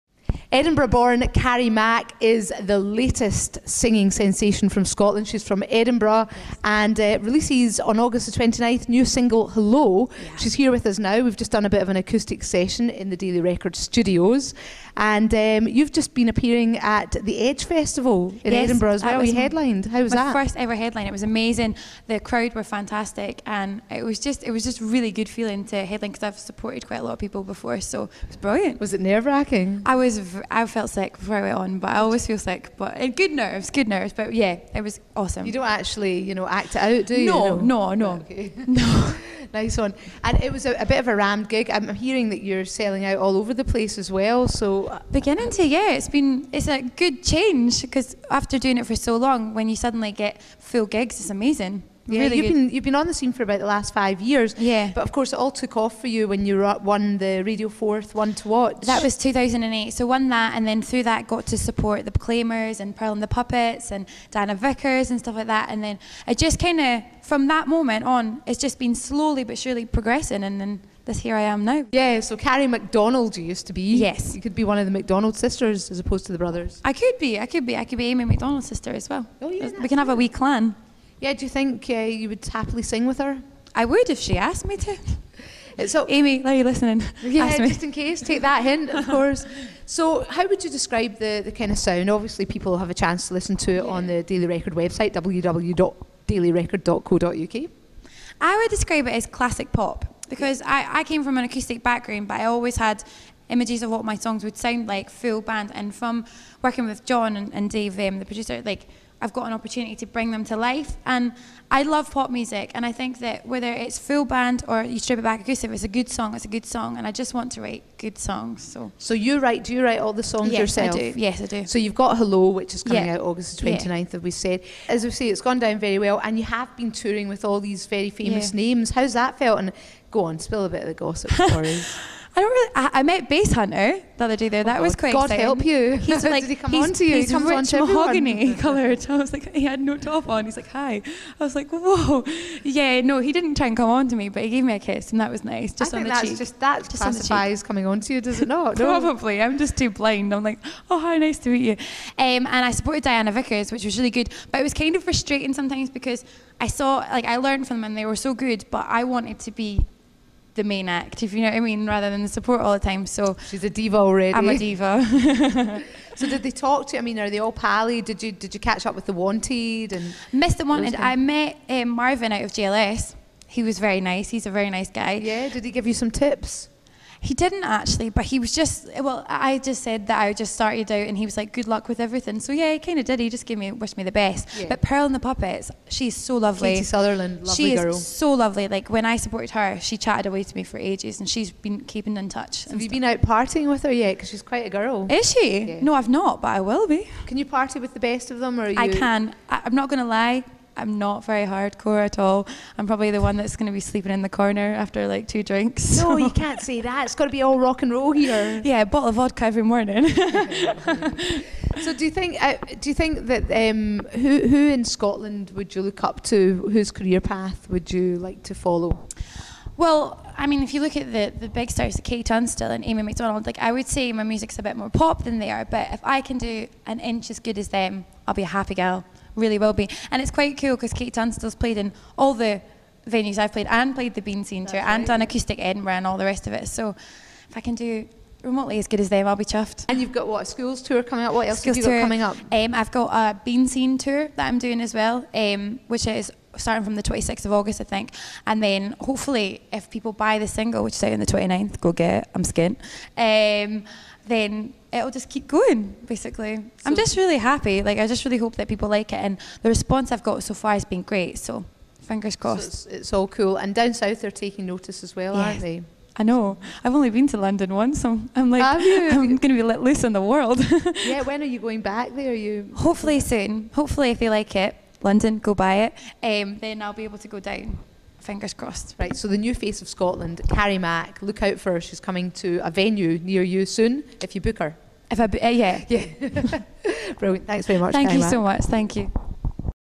Exclusive: Carrie Mac interview
Up-and-coming Scottish singer Carrie Mac speaks to Razz girl Beverley Lyons about her plans for the next year in this exlclusive chat for dailyrecord.co.uk